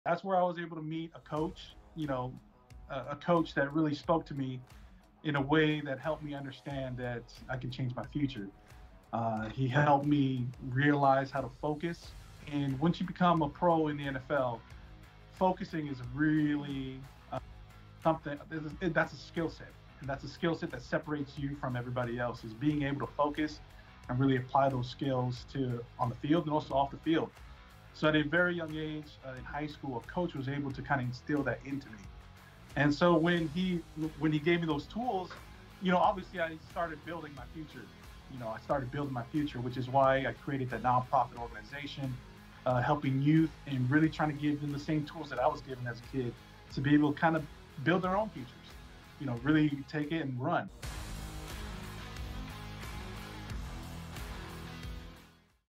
Pro NFL Advice: Learn How to Focus
Junior Aumavae, former NFL professional athlete, gives some advice to any aspiring NFL players: learn how to focus. This skillset proved invaluable in his career and set him apart from his peers.